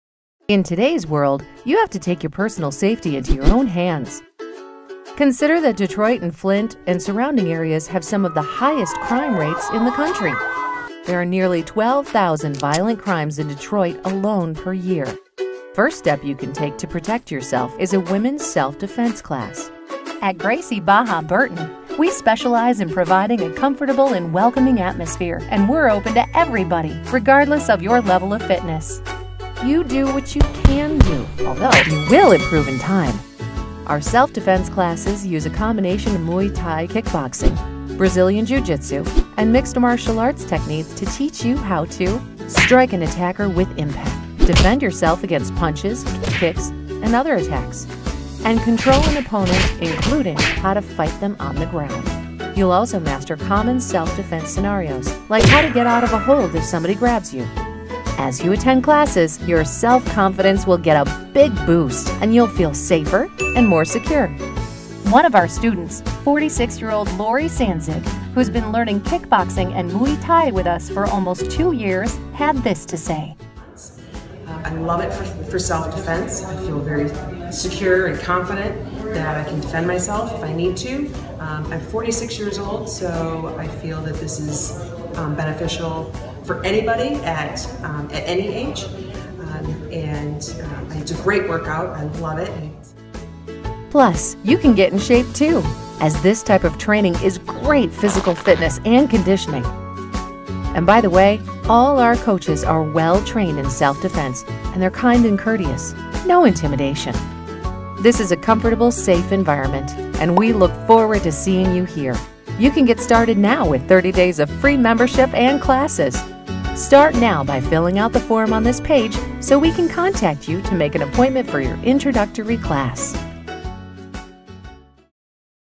Jim Burchfield Women's Self Defense Burton
In today's world, we have to take our own safety into our own hands. Our self-defense classes use a mixed approach to learning self-defense. You will learn certain scenarios and how to defeat each.

Visit adtoons.com to learn about other martial arts gyms and classes around the country